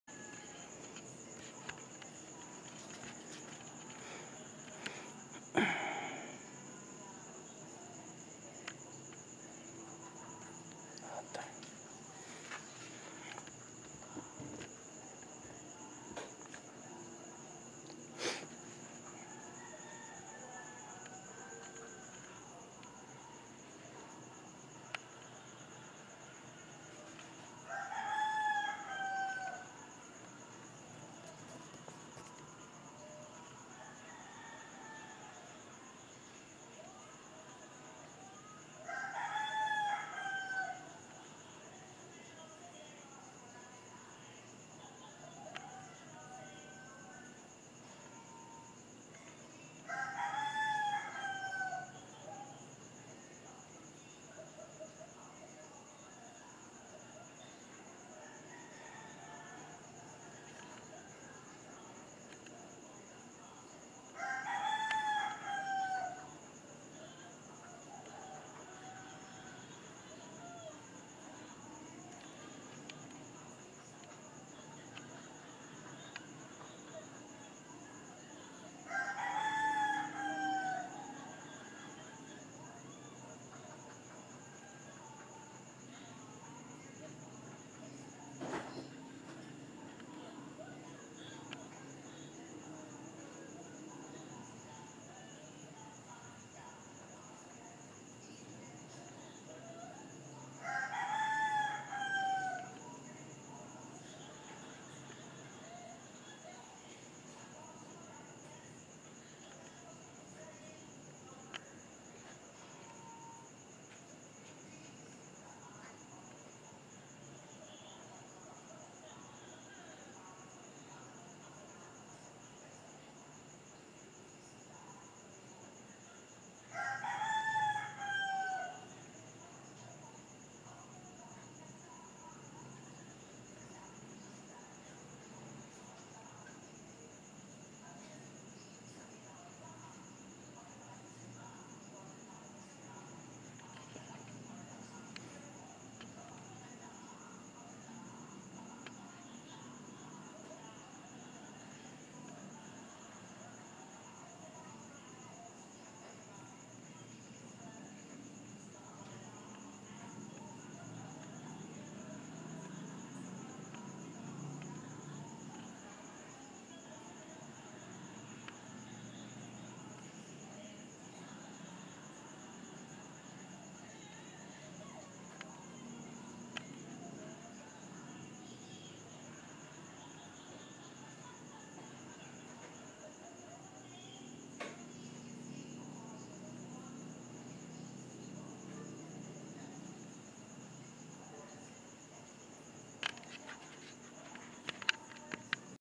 First attempt to shoot the moon
Canon Vixia HF R40 with 1060x digital zoom on.
Shot July 14, 2014, 1:17AM (Philippine Time).
Forgive the shakes, it was on a tripod, but on an awkward position.